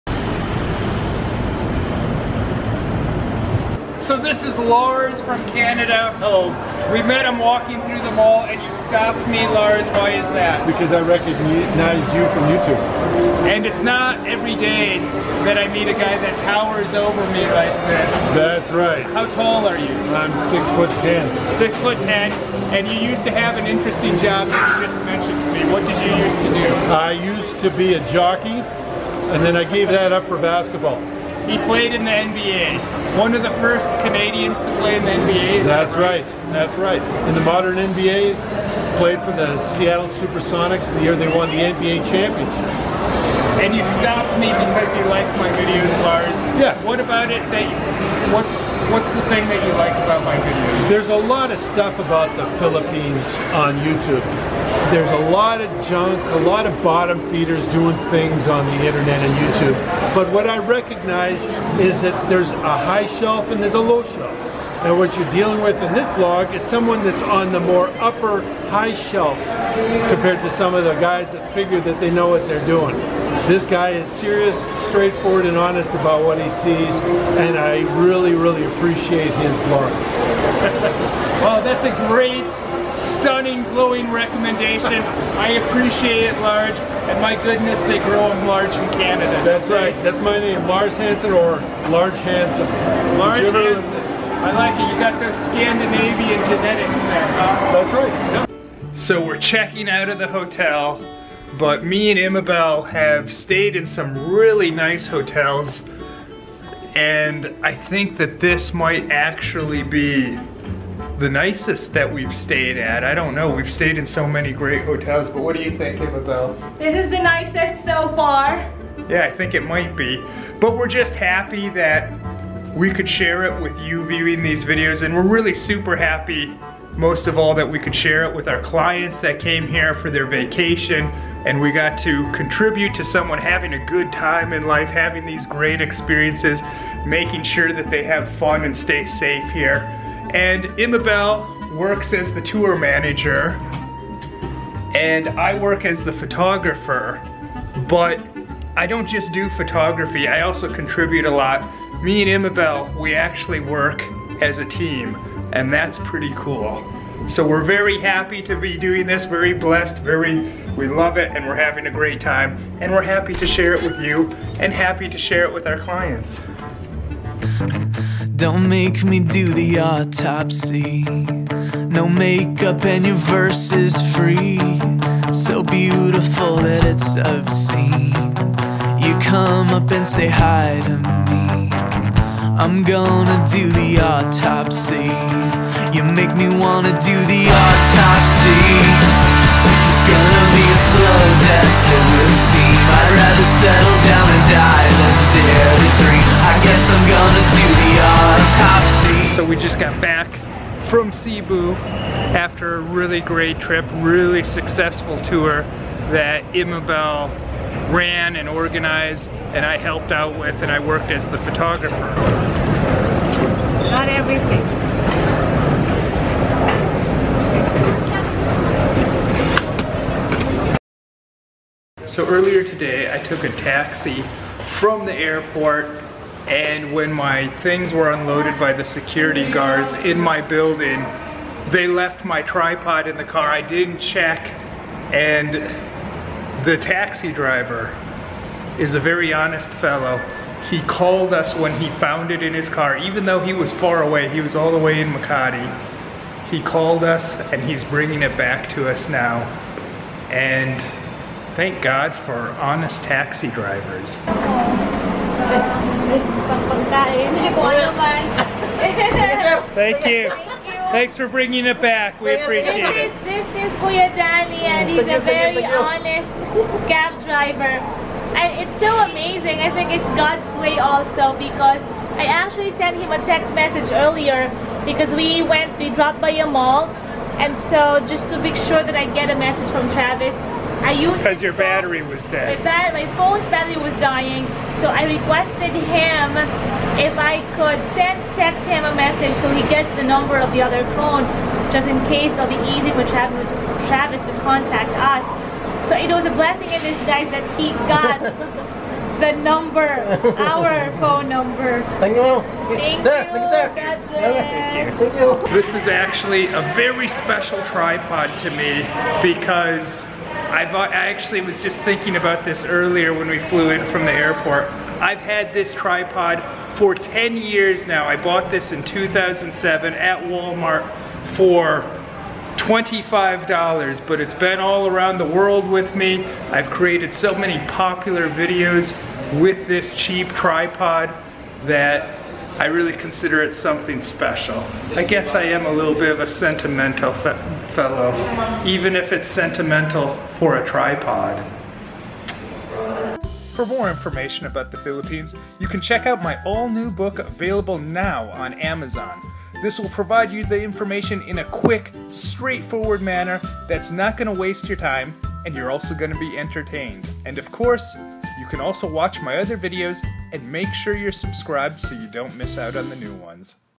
IMeeting a retired NBA Player in the Philippines - YOU HAVE TO SEE THIS GUY
➜ New Book
➜ Camera used to make this video
Cebu, Philippines - While at Ayala Center Cebu, I was stopped by a rather large fellow named Lars Hansen, a former NBA player from Canada. We have a lovely talk and then we check out of our amazing hotel and fly back to Manila. My tripod was left in the taxi but the honest taxi driver brought it back to me.